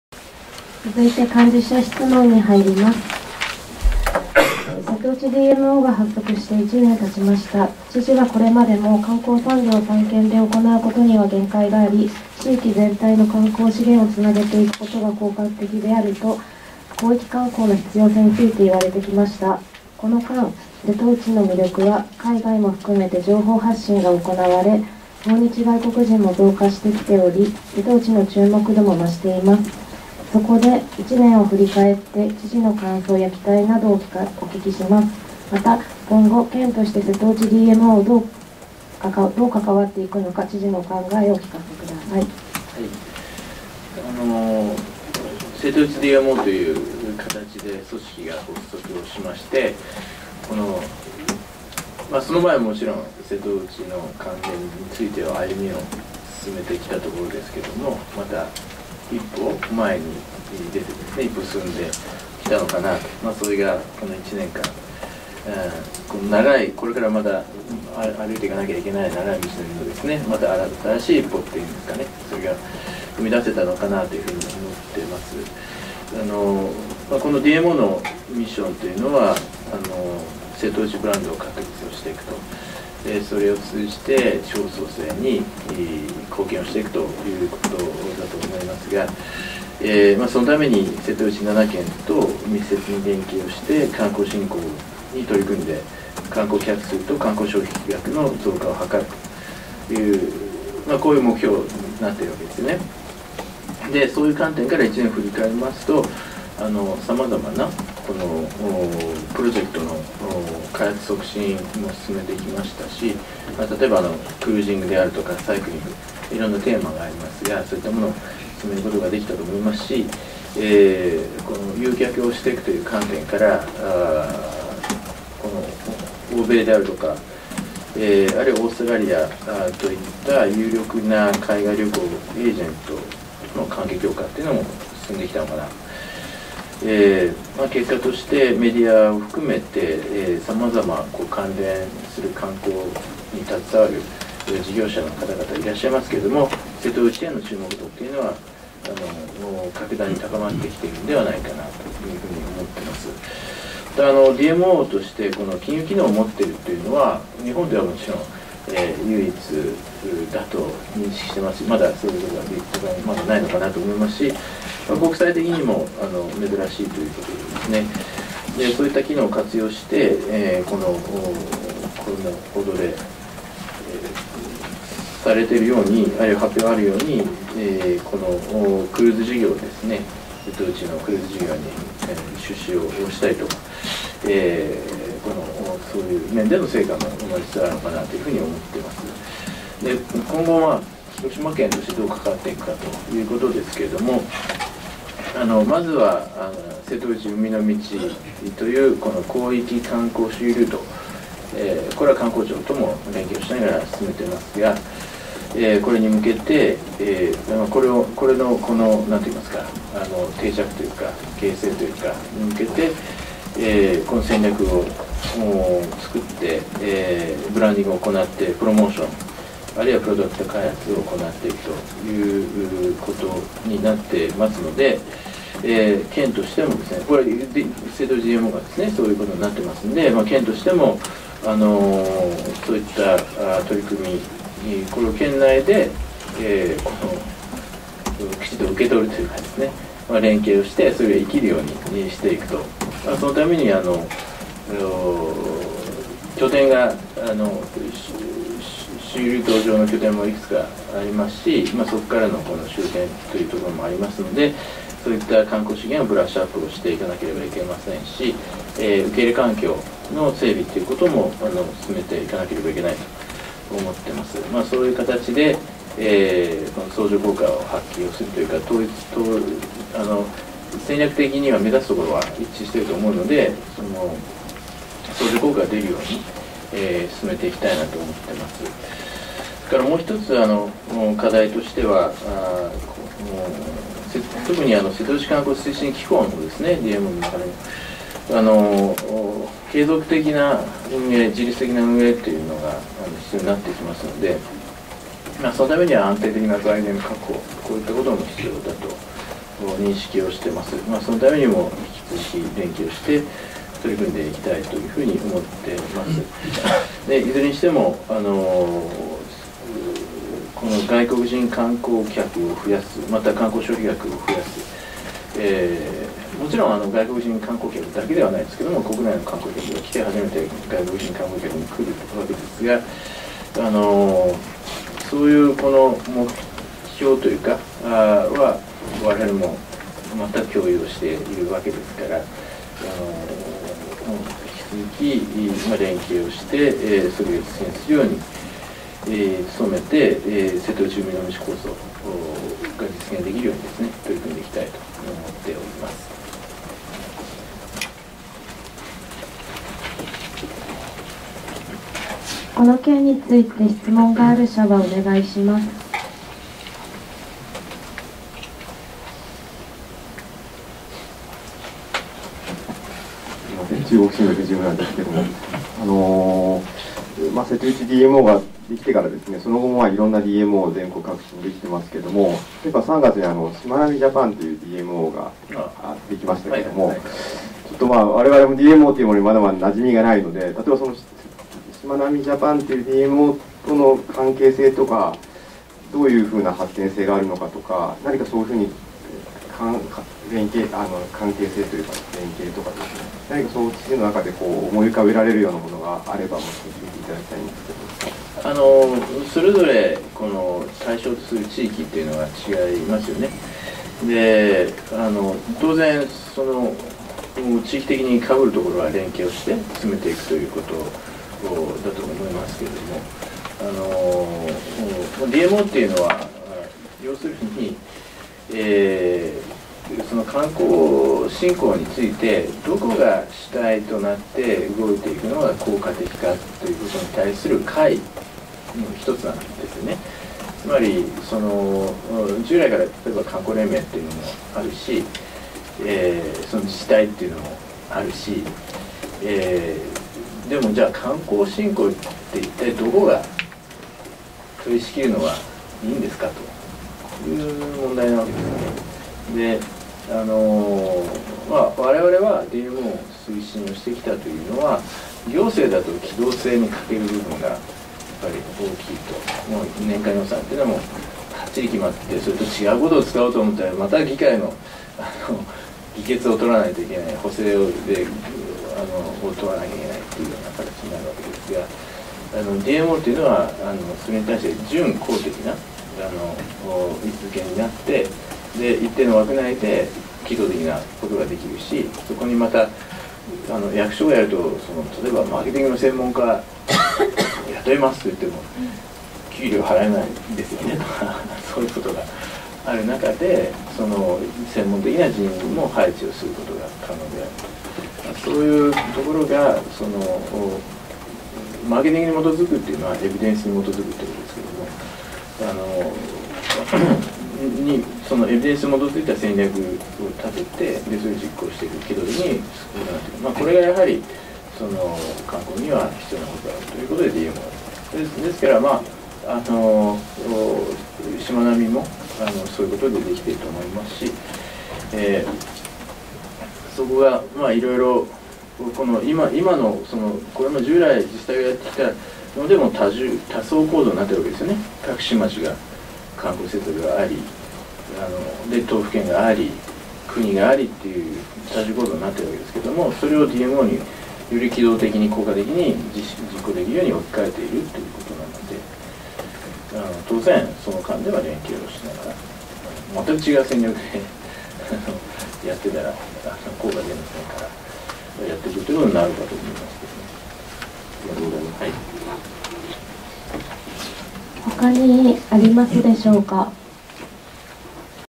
平成29年4月18日広島県知事会見 (質疑:「せとうちDMO」について)
ひろしまけんインターネットチャンネル